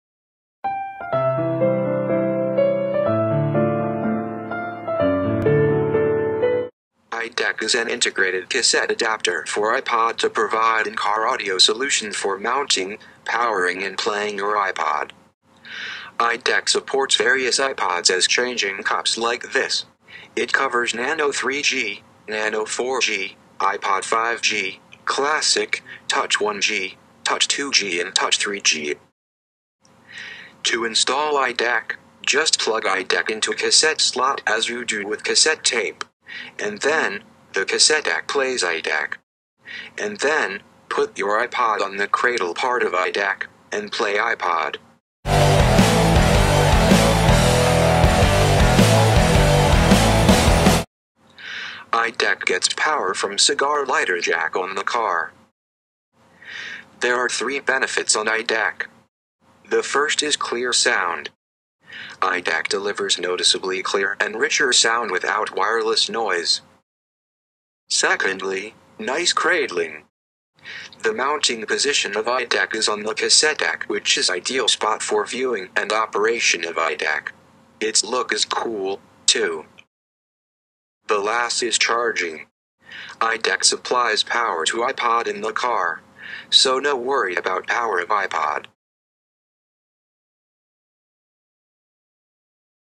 iDeck Review
iDeck converts your cassette deck into iPod audio system.